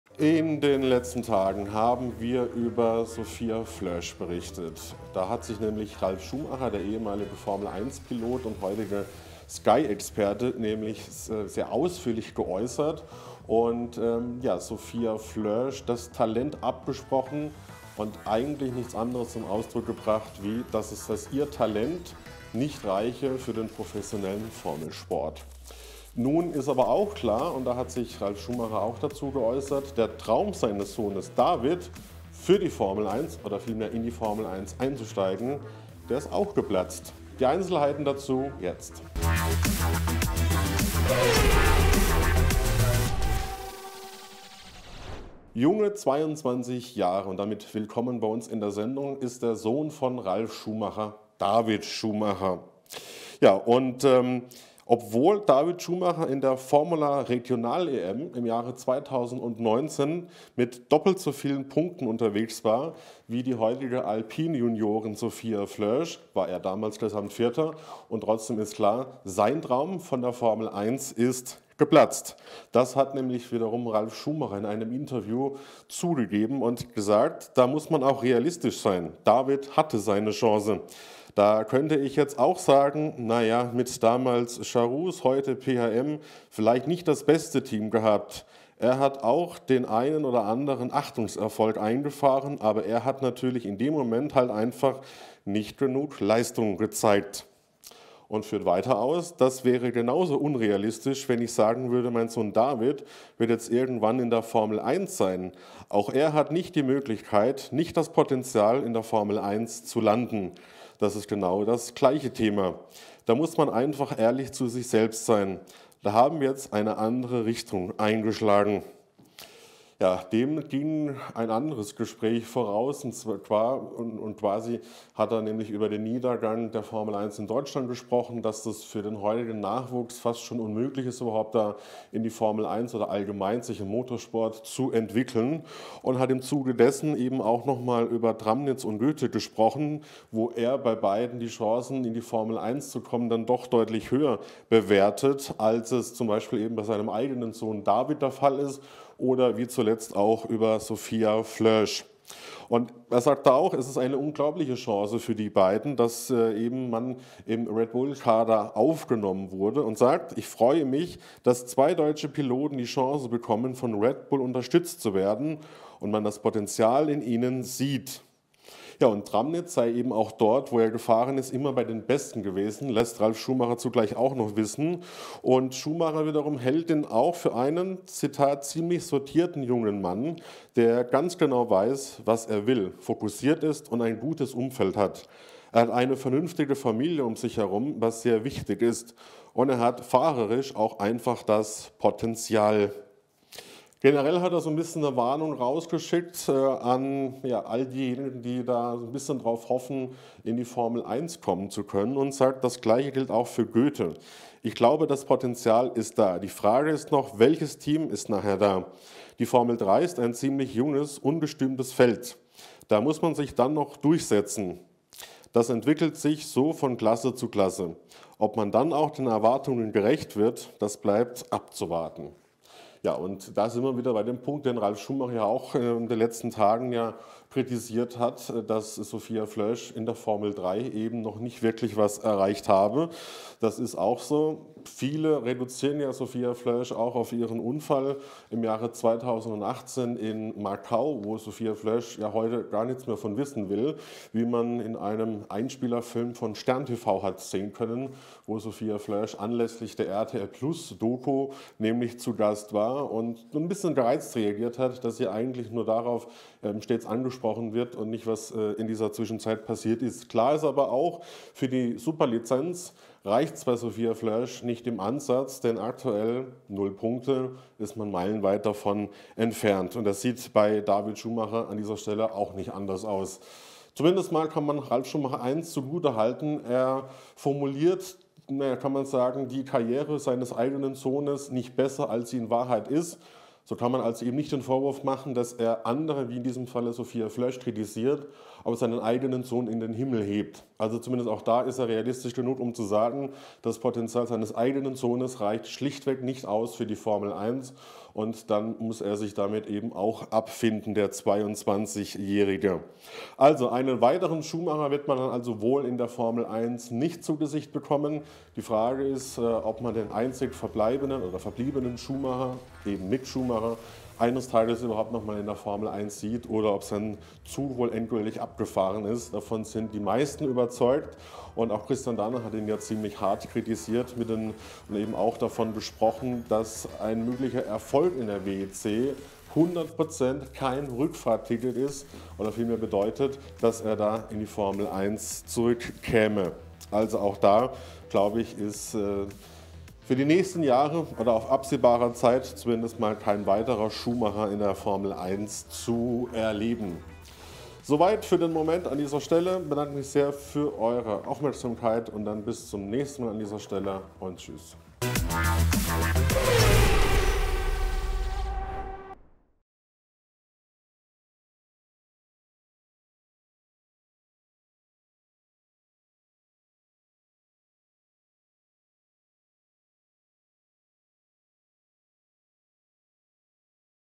Laut Ralf Schumacher hat sein Sohn David „nicht das Potenzial, in der Formel 1 zu landen“
Kanalmitglied werden und exklusive Vorteile erhalten:
Alle Infos zu Champ1, ganze Folgen und Formel 1-News gibt es
Beschreibung:

Es sind ehrliche Worte eines Vaters über seinen eigenen Sohn und zeigt: Nicht jeder Schumacher schafft den Sprung in die Königsklasse des Motorsports. Ralf Schumacher hat sich in einem Interview über den (nahezu fehlenden) Nachwuchs in Deutschland geäußert und dabei auch seinen eigenen Sohn David, junge 22 Jahre alt, dabei nicht außenvor gelassen.